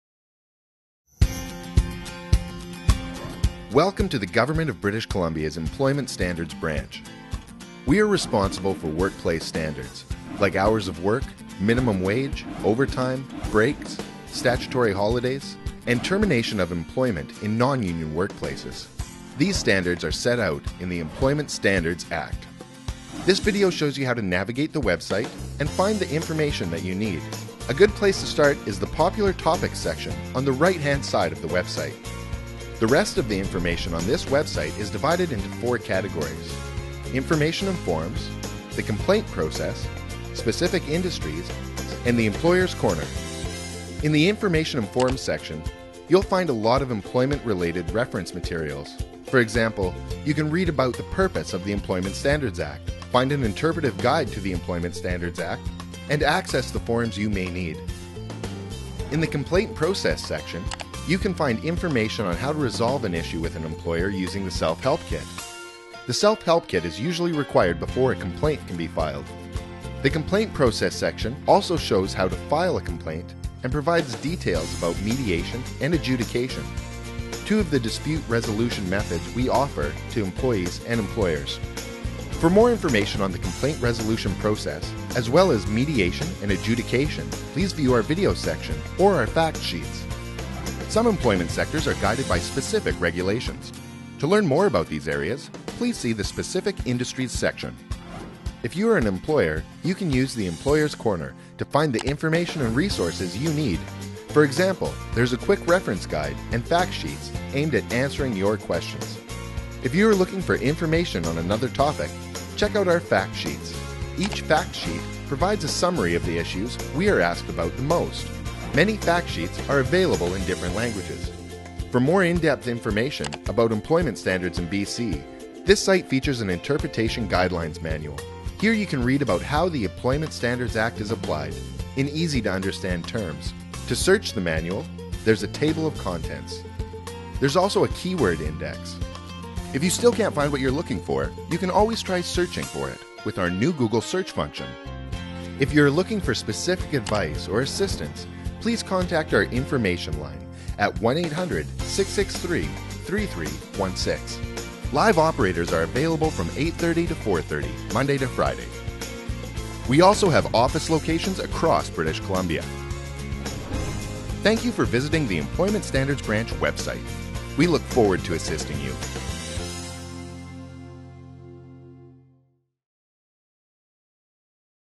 Navigating the British Columbia Employment Standards Branch Website
A video to help you find the tools and resources on the BC Employment Standards Branch Website.